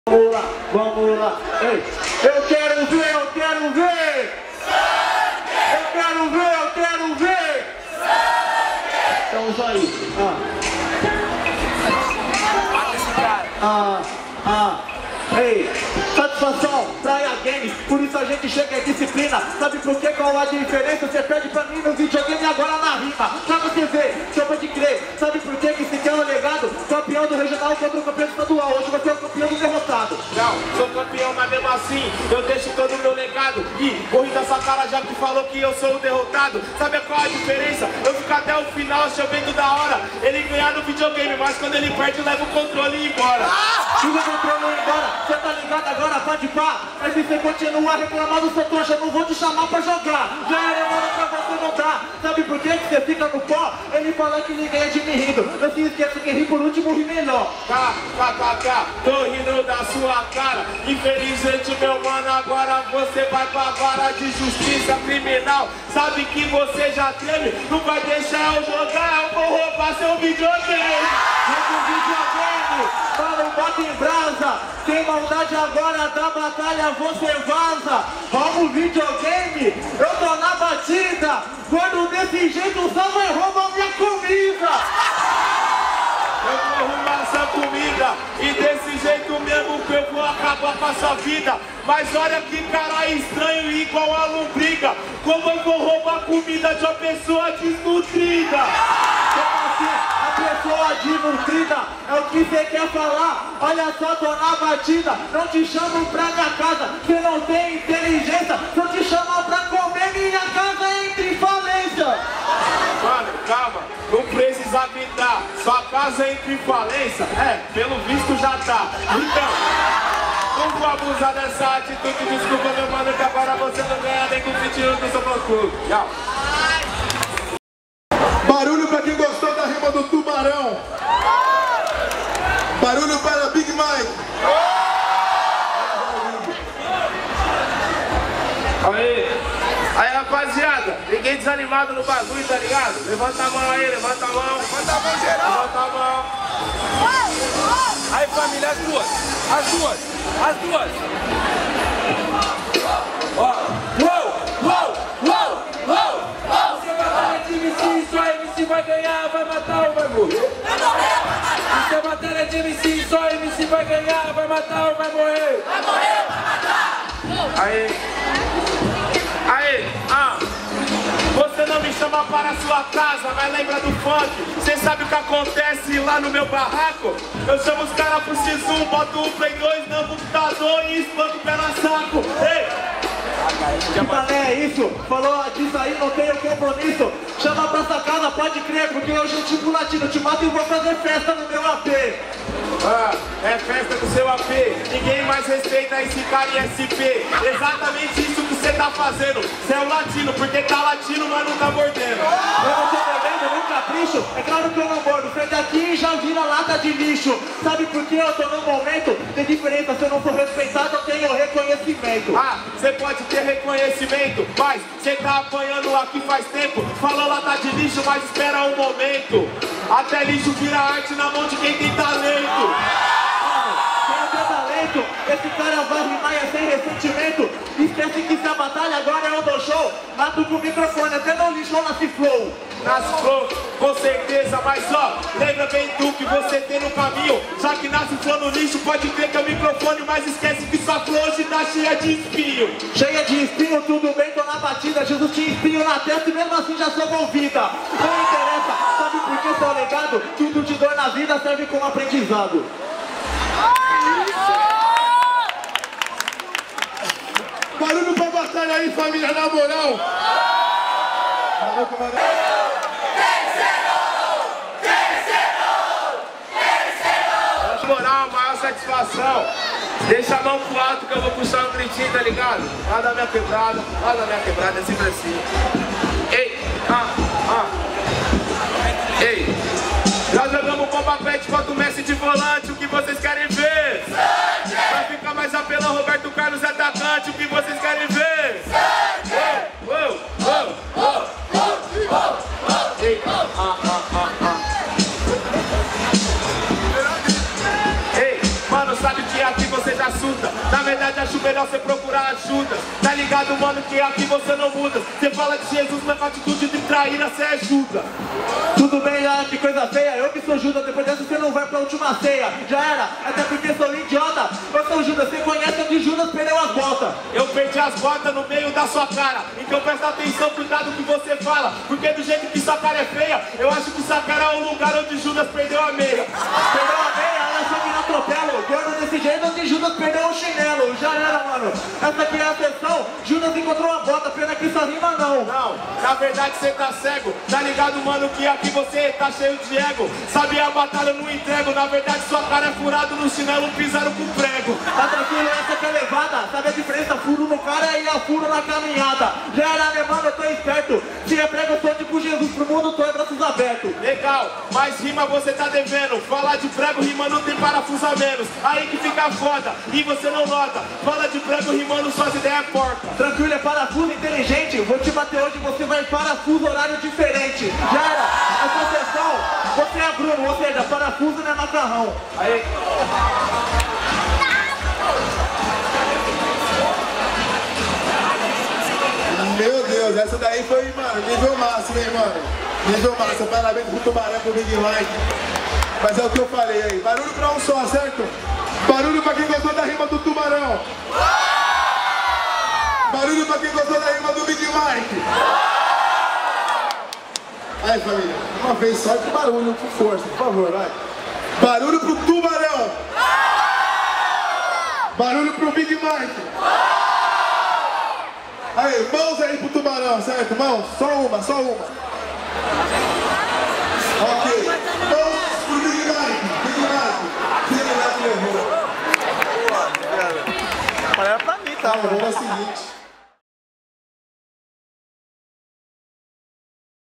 Big Mike x Tubarão | PRAIA GAMES | 14.09.2019
A Batalha da Aldeia foi convocada pelo Raillow  para colar no Praia Games, e mandamos nosso representante do E-Sports (Alvarenga) junto com 2 MC's para representar o time!

Rolou uma batalha lá, confere aí as rimas!!

Foi muito legal ter colado e sentido o calor de todos vocês da Praia Grande, até a próxima!!

Contrate a BDA: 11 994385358